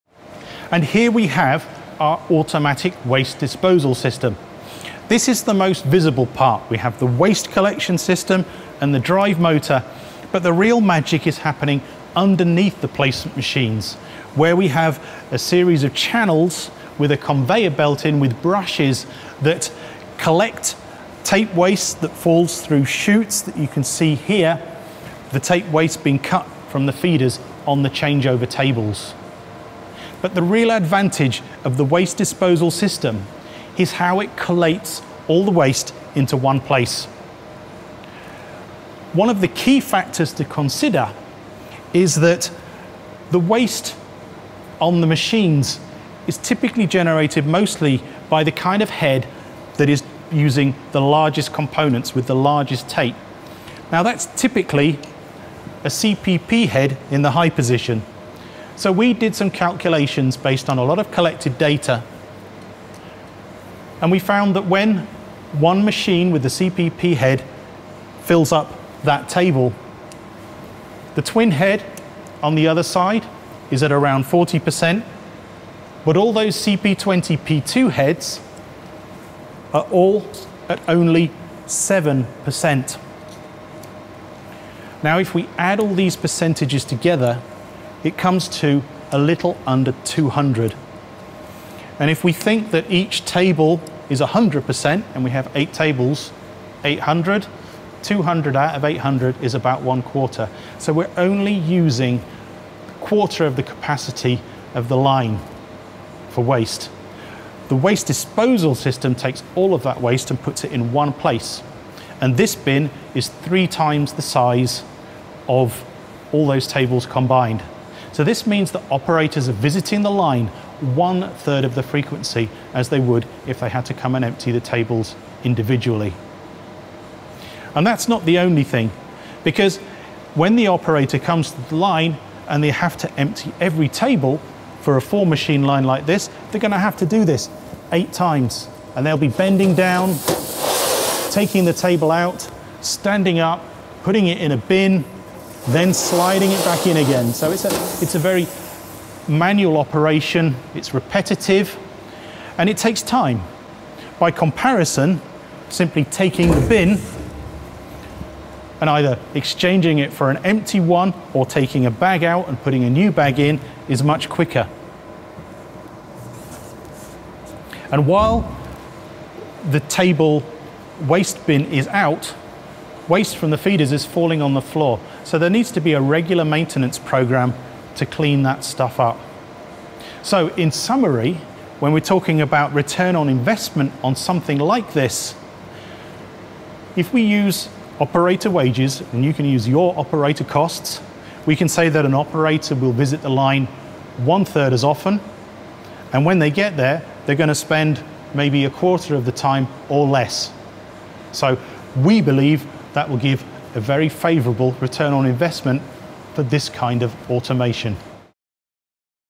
Facts on Open Automation | The 80/20 rule: All eyes on ASM Automatic Waste Disposal System
The 80/20 rule: Automating in a way that makes sense financially.

Automation? That’s a definite yes, but not at any price - learn how much automation really pays off.

We take a look at the concrete effects of automation in SMT production and use the Pareto Principle to explain how SMT manufacturers can invest in automation step-by-step in a way that makes financial sense. Enjoy a successful mix of hands-on examples and the concentrated knowledge of automation experts Alexander Hagenfeldt, Rob Raine and Kenneth Koh directly from the ASM Center of Competence.

This video shows our brand new Automatic Waste Disposal System.